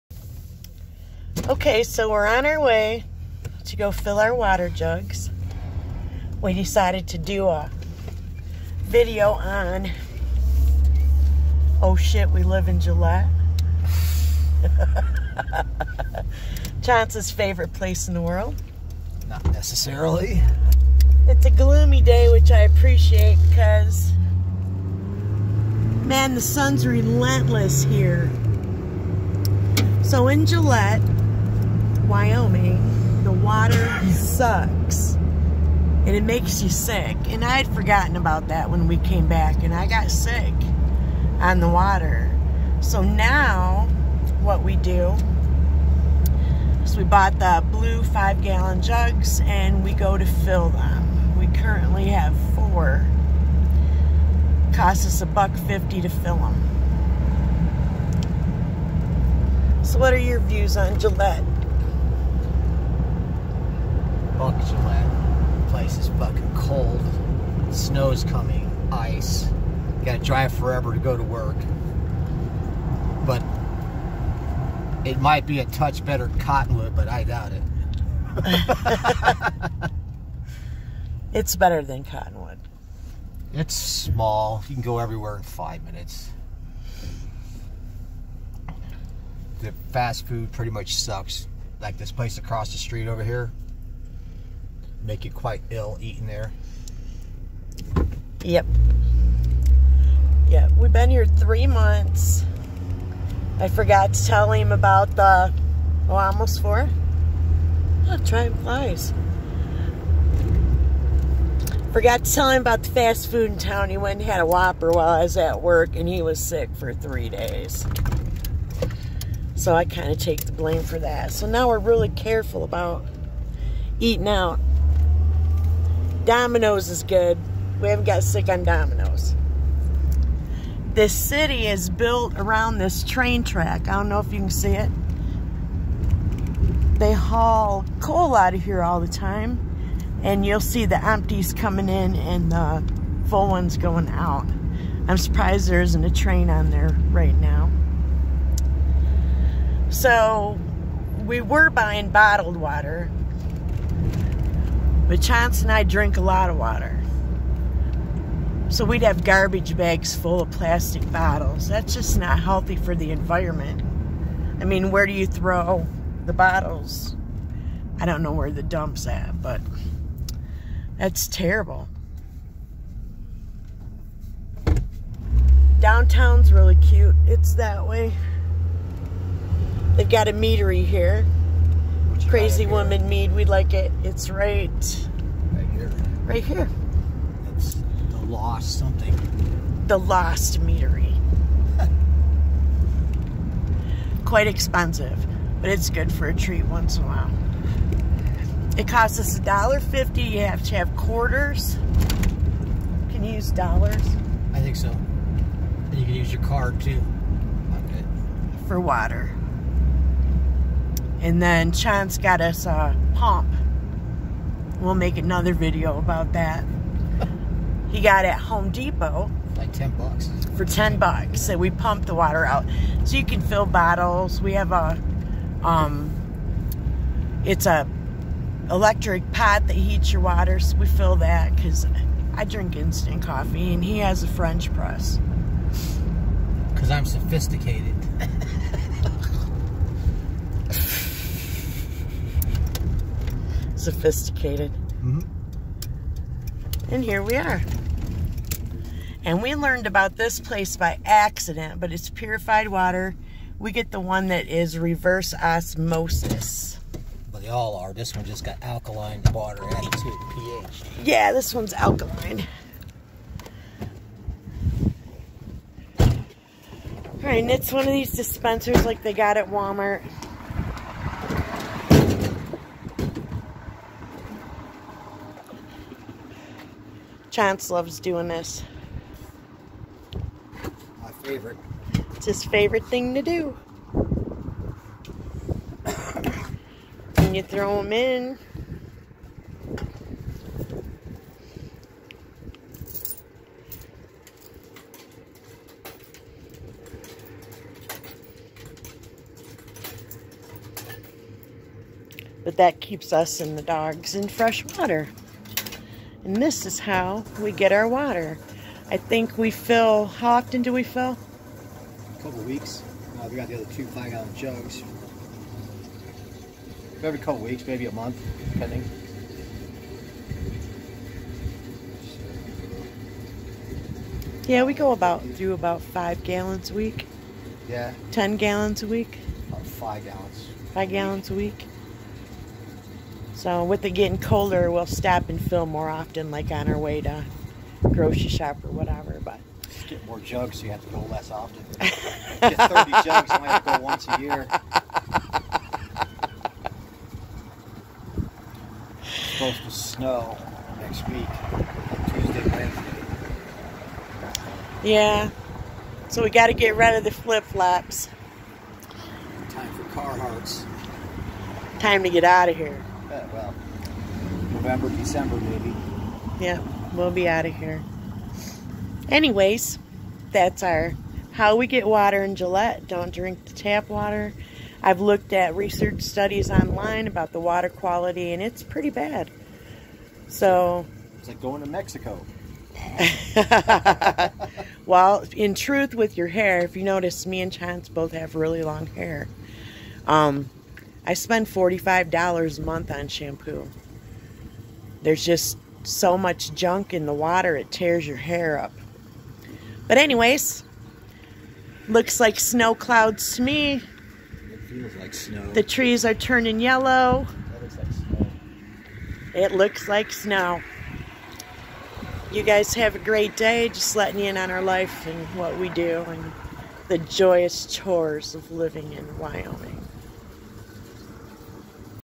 Getting Water in Wyoming
Just us filling our water jugs and speaking about Wyoming